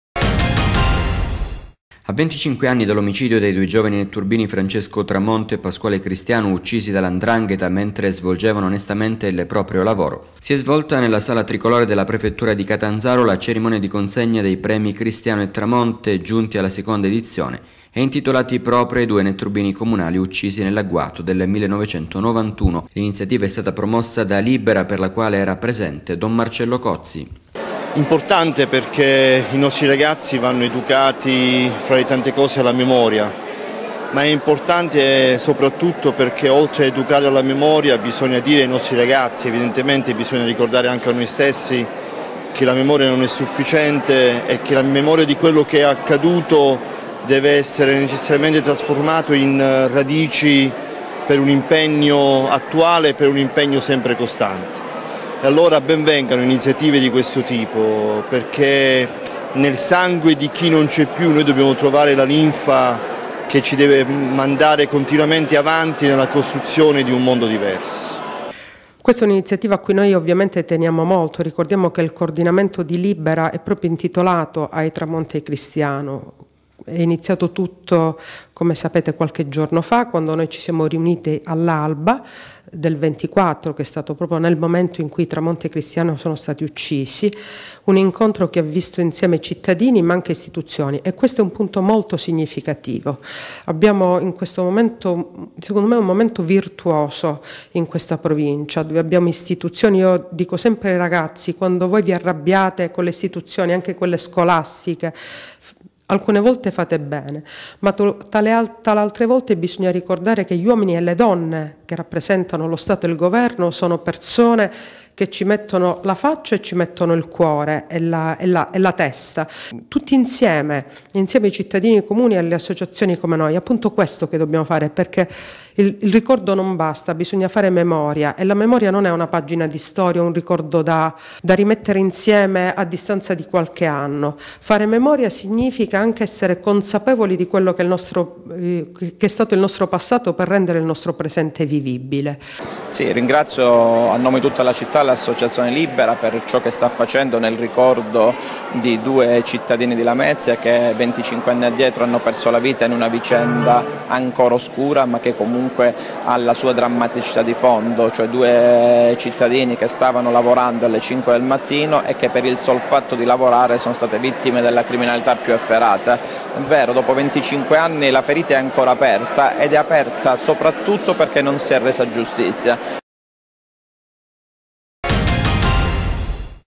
Cerimonia di consegna dei Premi Cristiano e Tramonte RTC TELECALABRIA
A 25 anni dall'omicidio dei due giovani netturbini Francesco Tramonte e Pasquale Cristiano, uccisi dalla 'ndrangheta mentre svolgevano onestamente il proprio lavoro, un grande valore non solo simbolico ma sociale, politico, storico. "Dobbiamo dire grazie - riporta ancora il comunicato - al coordinamento Libera Catanzaro che ha permesso che la storia di due lavoratori, vittime innocenti di ndrangheta, non fosse relegata nel dimenticatoio; la Cgil chiede verità e giustizia civile e sociale per le vittime, i familiari e per l'intera società civile. I collaboratori lametini che aiutano con le loro dichiarazioni la magistratura, contribuendo alla scoperta di episodi e fatti criminali che hanno riguardato la storia di Lamezia Terme, si ricordino di quanto accaduto 25 anni fa. Il sindacato, insieme agli altri soggetti che si battono per la verità, farà la propria parte perché la giustizia prevalga sullo strapotere e sulle logiche oscure che hanno influito ed influiscono pesantemente sulle nostre vite e sul bisogno di riscatto e di emancipazione della nostra terra. Interviste a Marcello Cozzi (vicepresidente naz. "Libera"), Donatella Monteverdi (referente prov. "Libera" Catanzaro), Paolo Mascaro (sindaco di Lamezia Terme)